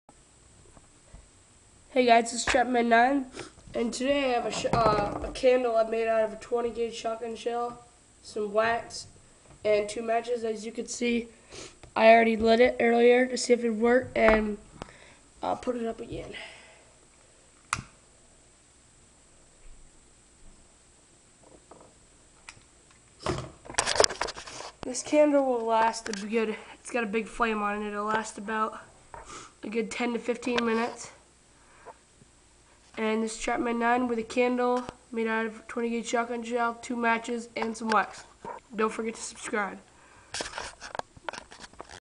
Outdoor Survival: Shotgun Shell Candle
Me making a candle out of; 2 matches, some used wax, and a empty 20 guage shotgun shell.